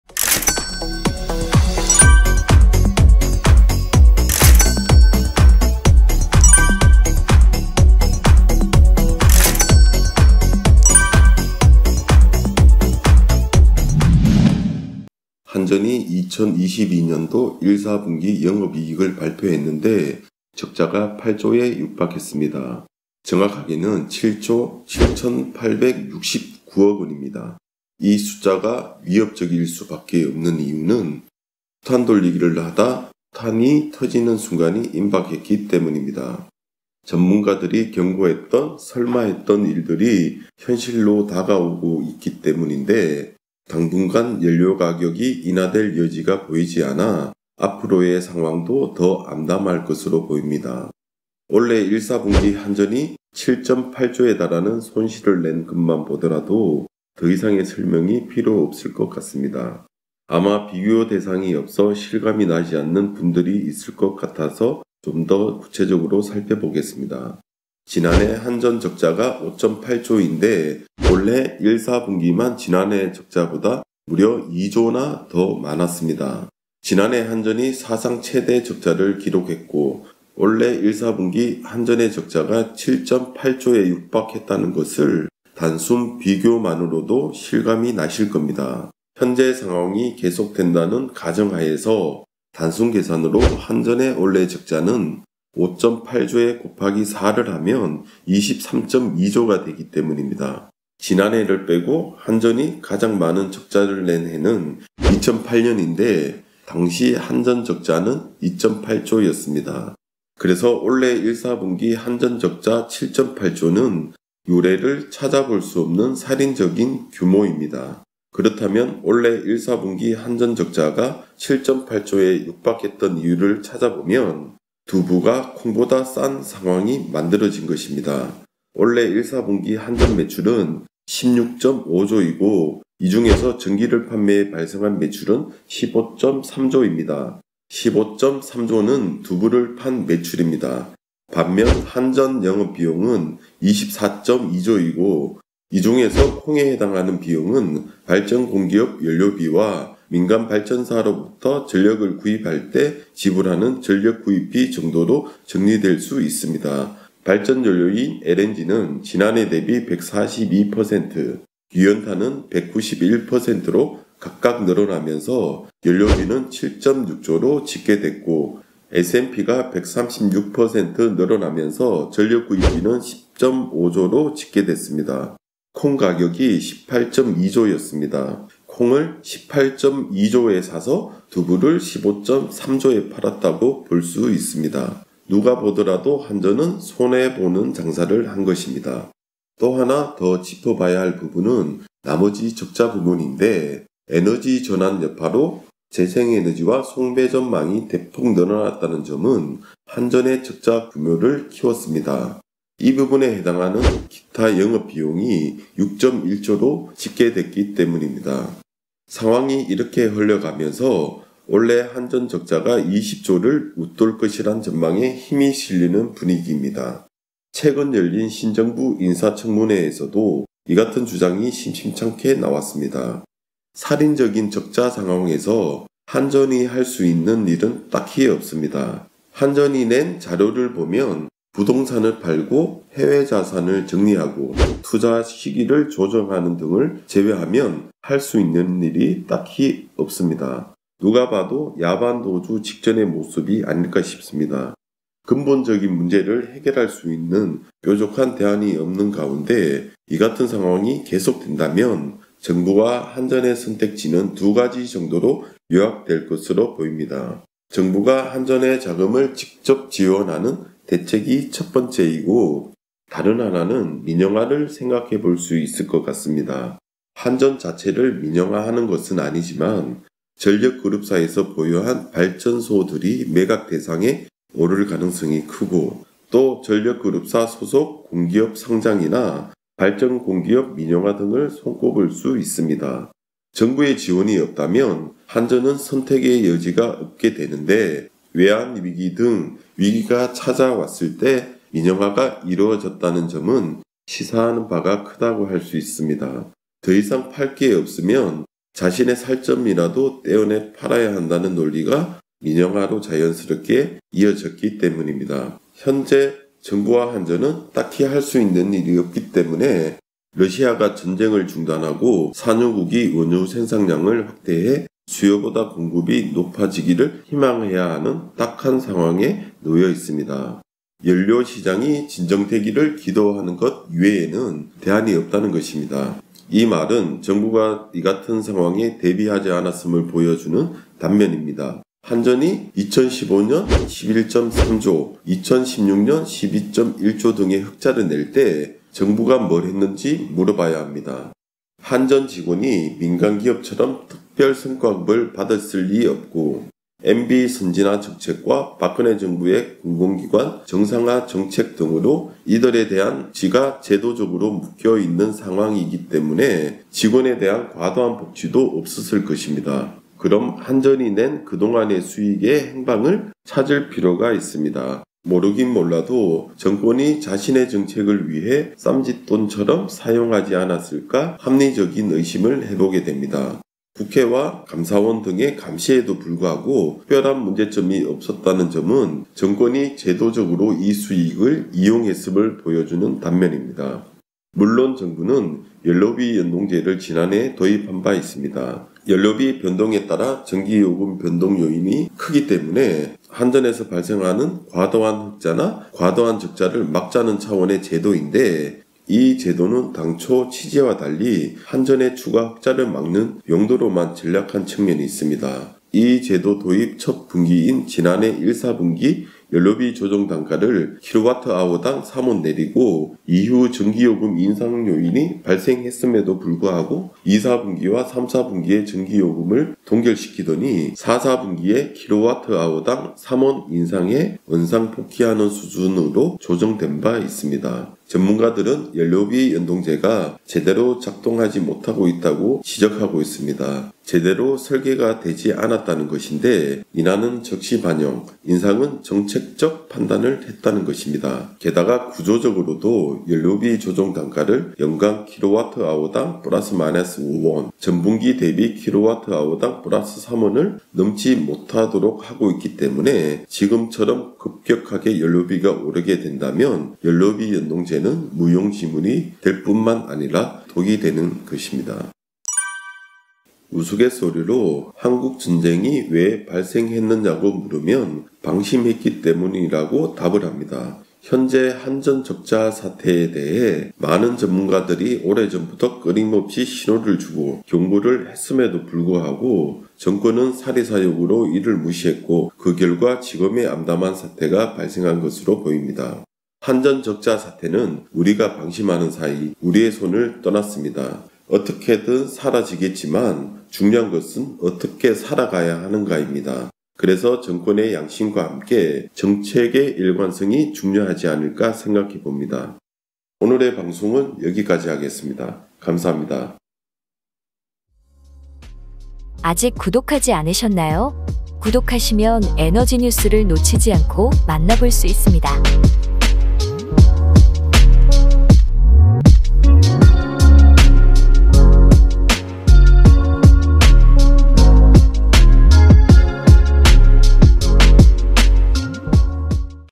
한전 이대로라면 살점 떼어내는 민영화?
한전이 2022년도 1/4분기 영업이익을 발표했는데 적자가 8조에 육박했습니다.
올해 1/4분기 적자가 최대 적자를 기록했던 지난해 적자보다 무려 2조나 더 많았습니다.
올해 한전 적자가 20조를 웃돌 것이란 전망에 힘이 실리는 분위기입니다.
최근 열린 신정부 인사청문회에서도 이 같은 주장이 심심찮게 나왔습니다.
이 같은 상황이 계속된다면 정부와 한전의 선택지는
2가지 정도로 요약될 것으로 보입니다.
정부가 한전에 자금을 직접 지원하는 대책이 첫 번째이고, 다른 하나는 민영화를 생각해 볼 수 있을 것 같습니다.